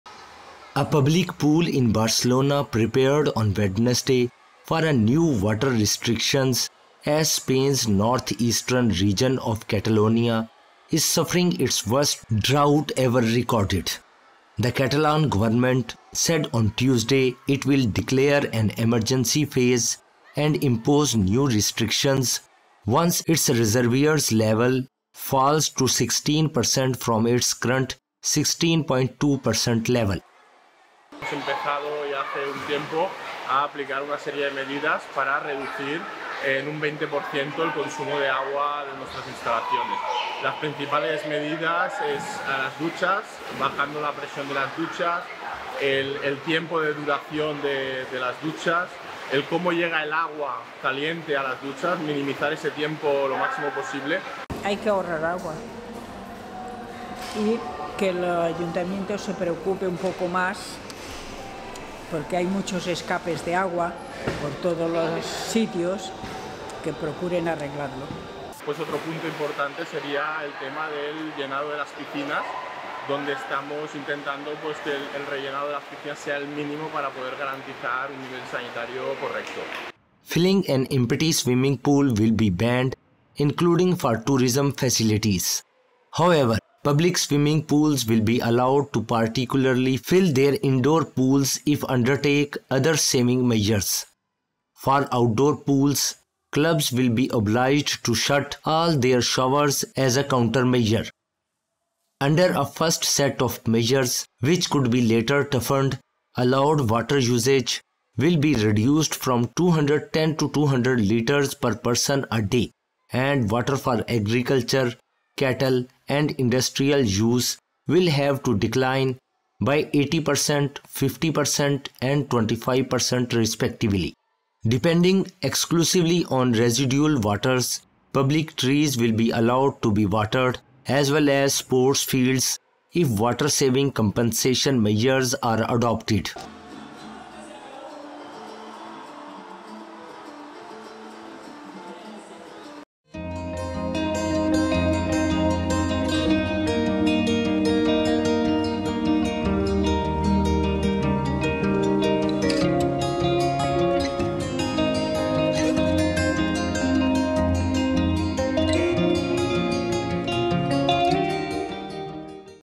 Barcelona braces for water restrictions as region suffers severe drought
A public pool in Barcelona prepared on Wednesday (January 17) for new water restrictions as Spain’s northeastern region of Catalonia is suffering its worst drought ever recorded.
The Catalan government said on Tuesday (January 16) it will declare an emergency phase and impose new restrictions once its reservoirs’ level falls to 16% from its current 16.2% level.
Filling an empty swimming pool will be banned, including for tourism facilities.
However, public swimming pools will be allowed to partially fill their indoor pools if undertake other saving measures.
For outdoor pools, clubs will be obliged to shut all their showers as a countermeasure.
Under a first set of measures which could be later toughened, allowed water usage will be reduced from 210 to 200 liters per person a day and water for agricultural, cattle and industrial use will have to decline by 80%, 50% and 25% respectively.
Depending exclusively on residual waters, public trees will be allowed to be watered, as well as sports’ fields if water saving compensation measures are adopted.